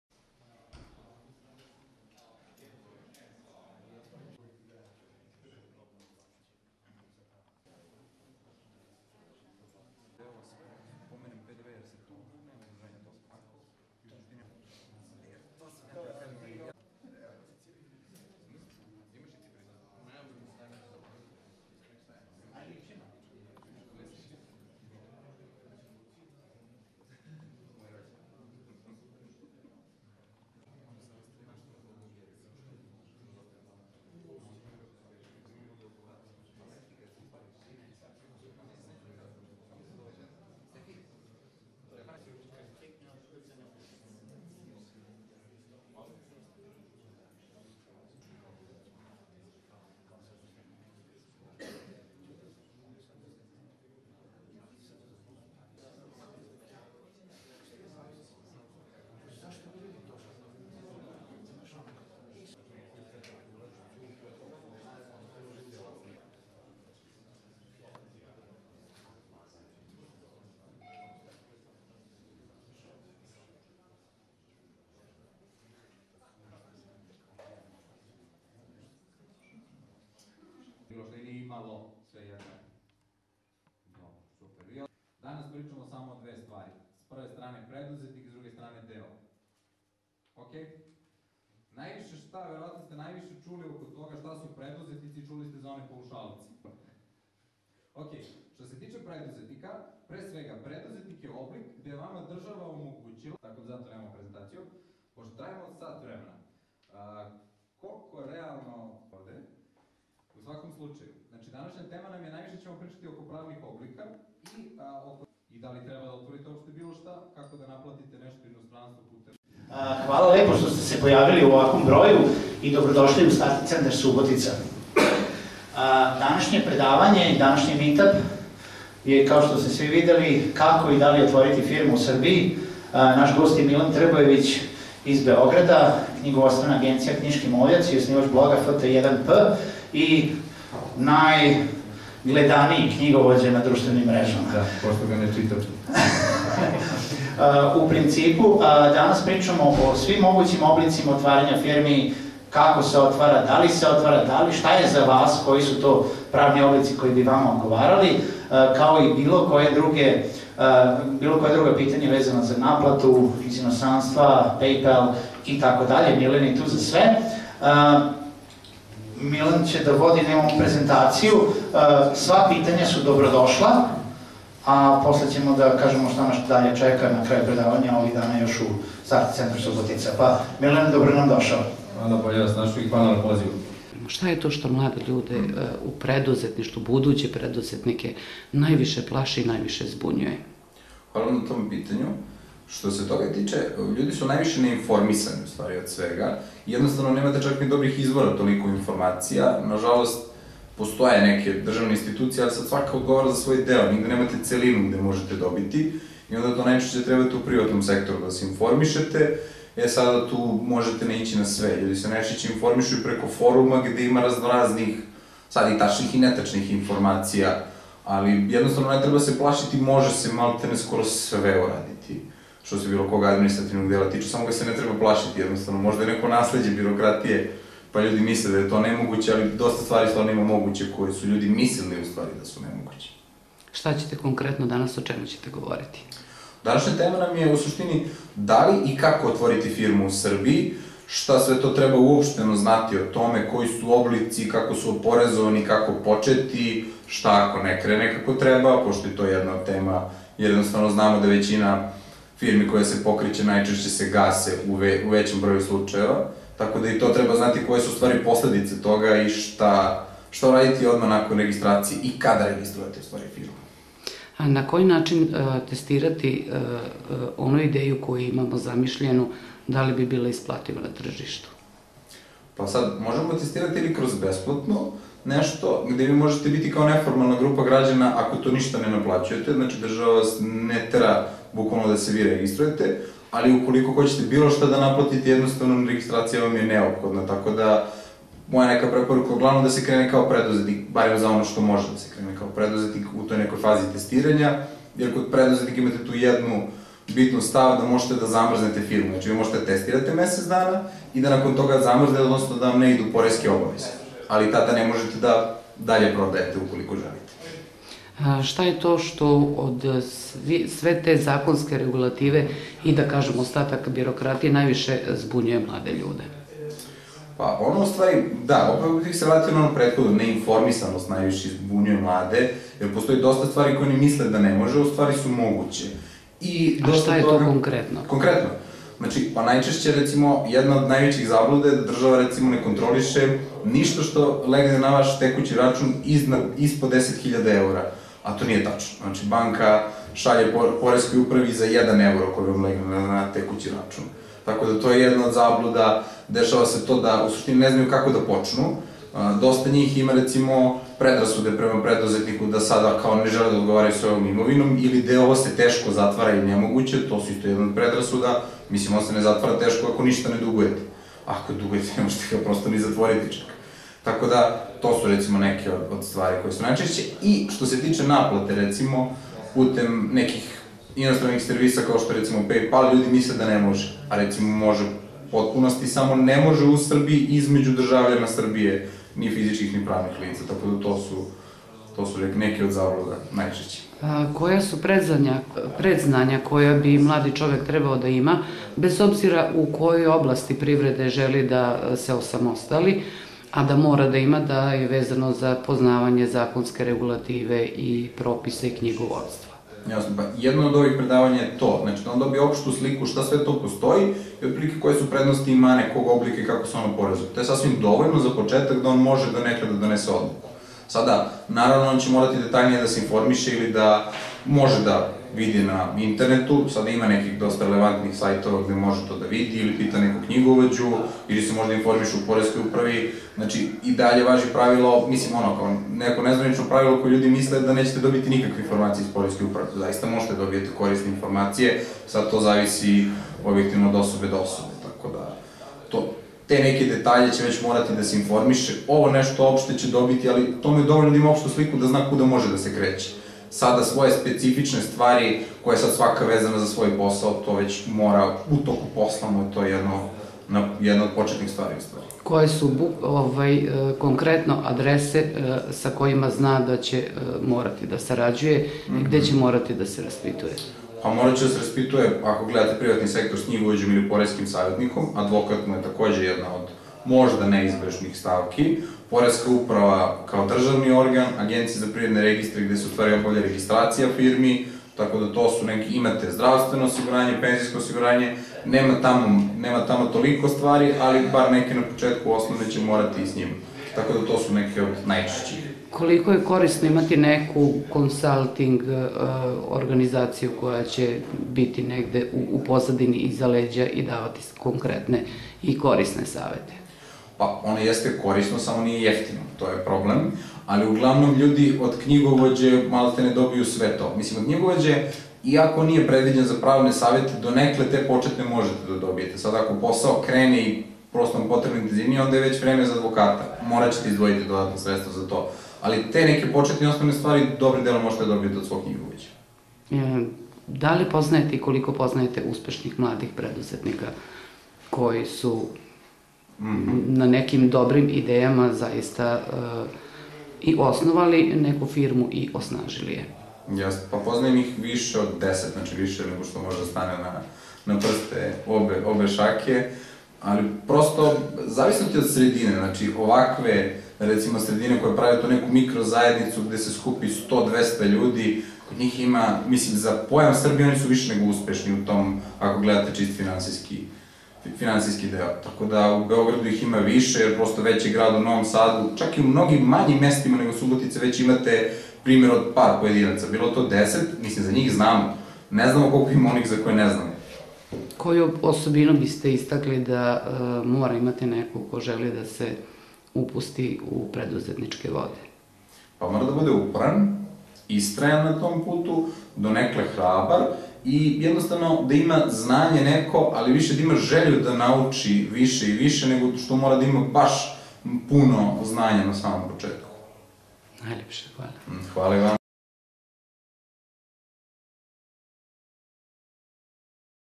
Milan Trbojevic Kako i da li osnovati firmu u Srbiji
super-info.rs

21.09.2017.

Održano predavanje na temu "Kako i da li osnivati firmu u Srbiji"

U Infostud hub i Startit centru u Subotici gostujući predavač Milan Trbojević iz knjgovodstvene agencije "Knjigovodstvo Knjiški moljac" iz Beograda, predstavio je sve začkoljice koje se mogu očekivati kod uplovljavanja u preduzetničke vode.

Trbojević je okupljenima predstavio sve pojedinosti vezane za otvaranje firme i njenim oblicima, a u posebnom razgovoru za naš portal je istakao i koje bi osobine morao imati budući mladi preduzetnik/preduzetnica.